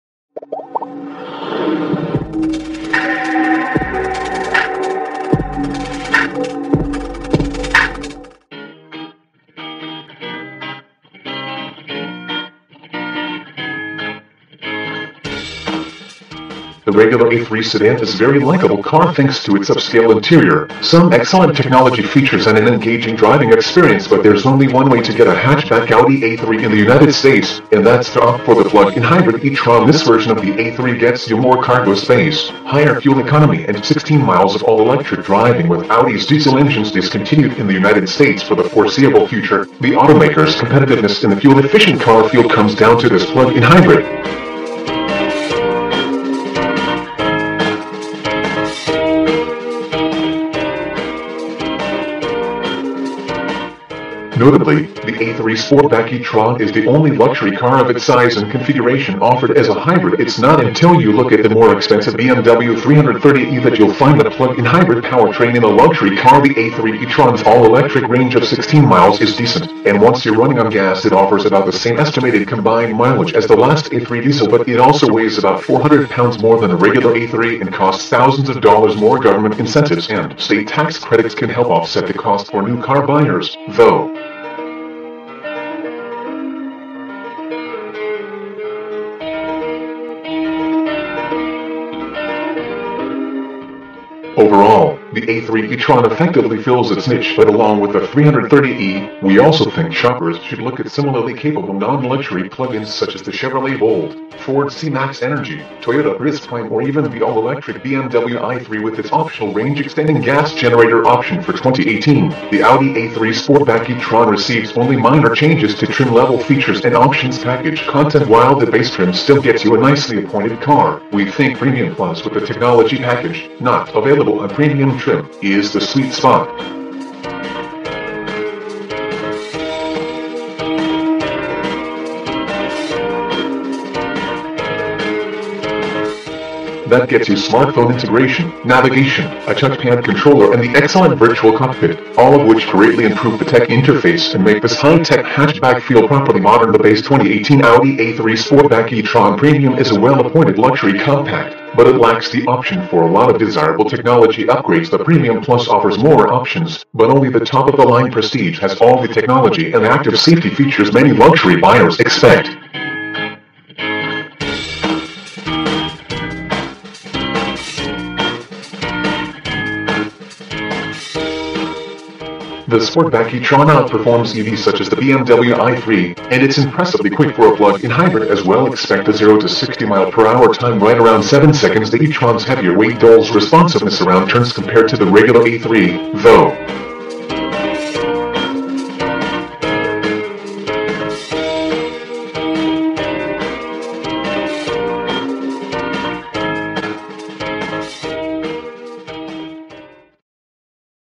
AMAZING !! 2018 Audi A3 E Tron
2018 Audi A3 E Tron

The regular A3 sedan is very likable car thanks to its upscale interior, some excellent technology features and an engaging driving experience. But there's o... . Video Title 2018 Audi A3 E Tron REVIEW .